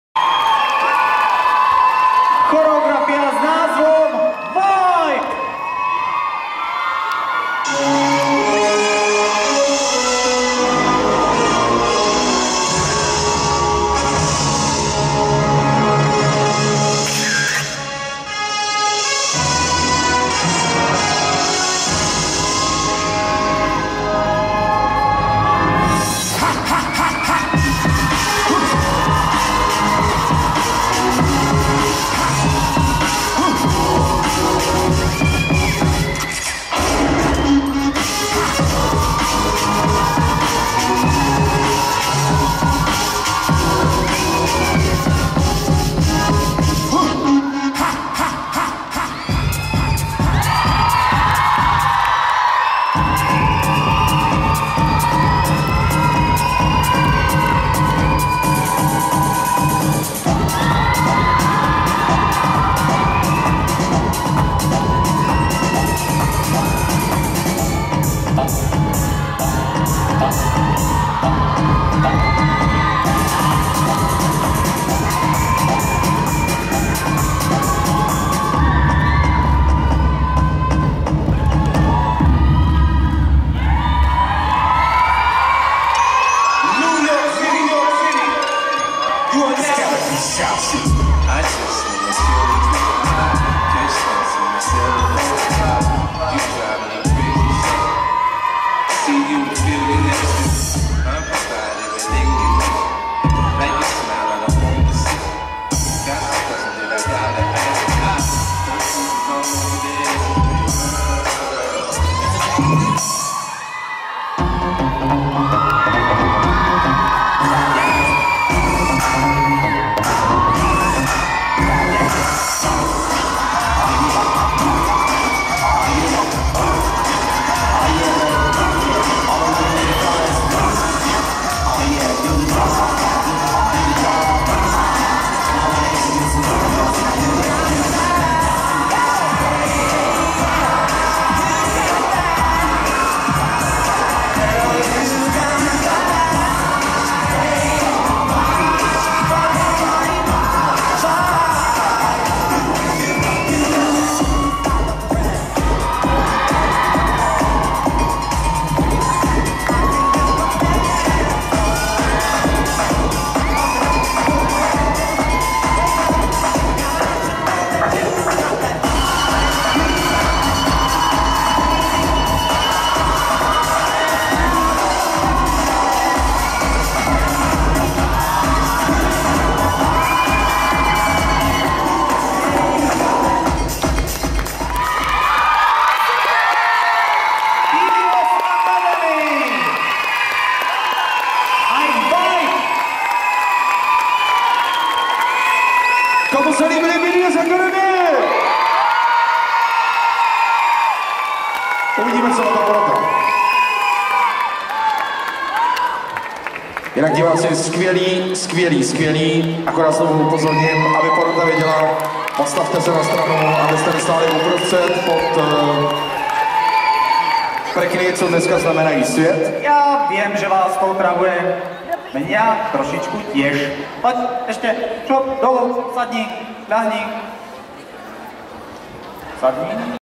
B.D.S academy taneční soutěž street dance juniori 7.4.2012
Vítězná formace pražského kola taneční soutěže